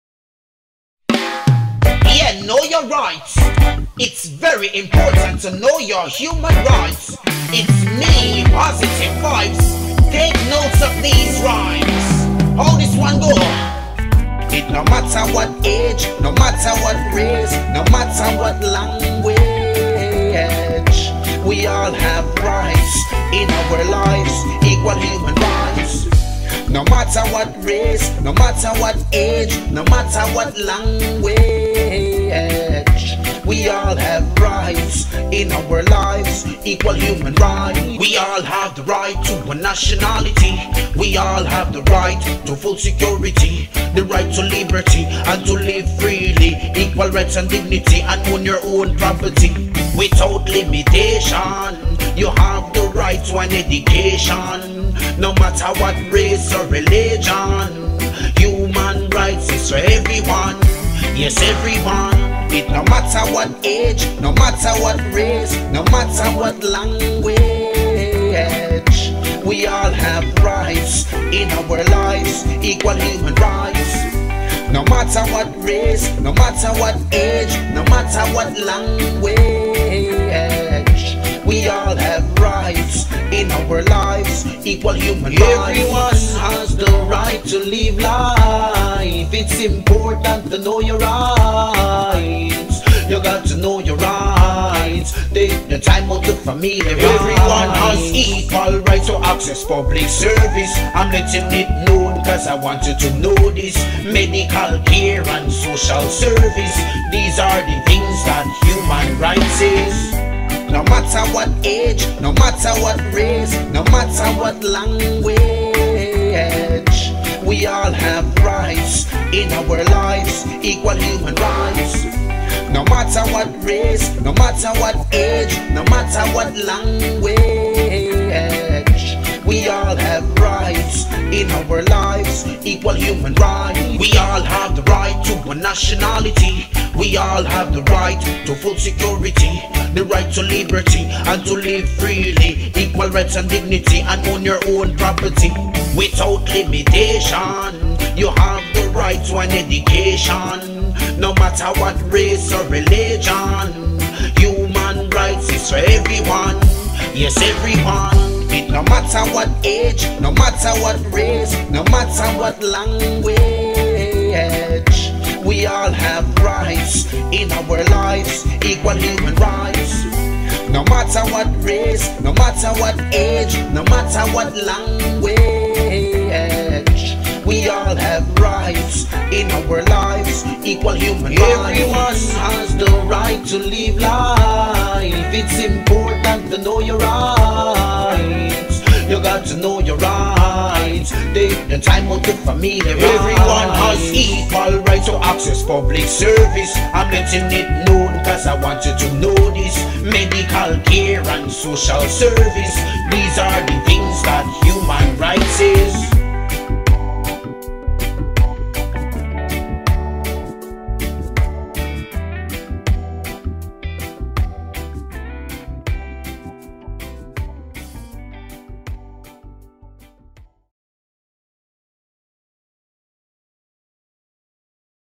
Positive Vibes Human Rights Music Video
A Music Video for the Human Rights Commission in Belize City.

Artist-Postive Vibes
Filmed/Edited-PreMedia Solutions
Directed-Zebedee Pitterson
Producer-Belize Music Agency-James Sanker
Commissioned by-Human Rights Commission